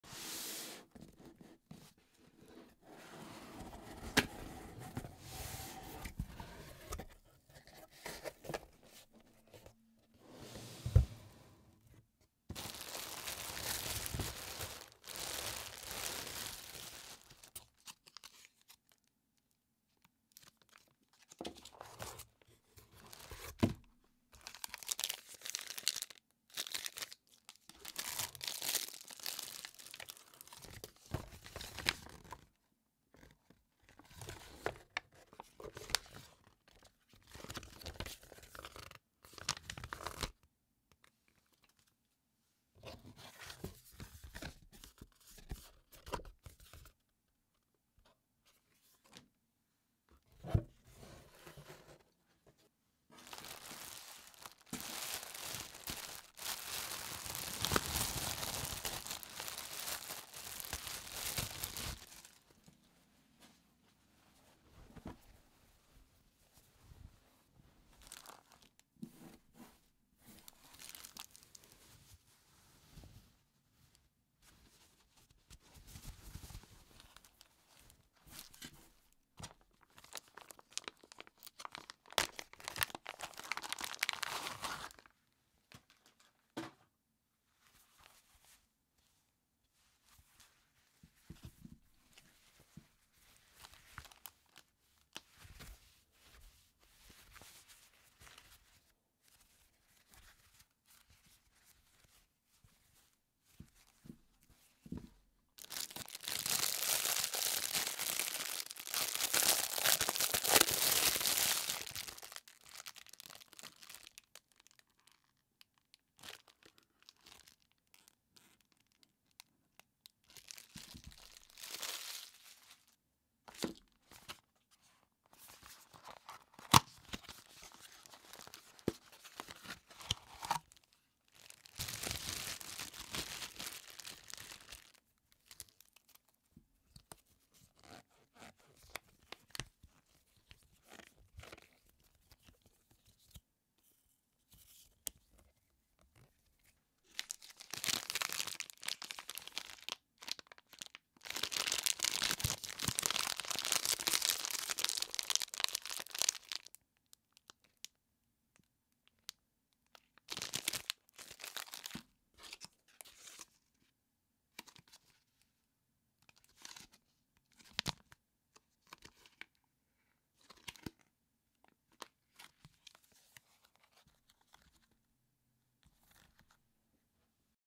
Unboxing April 2021 Gainzbox: Agility Edition
Unboxing the April 2021 Gainzbox featuring agility forward equipments and some snacks!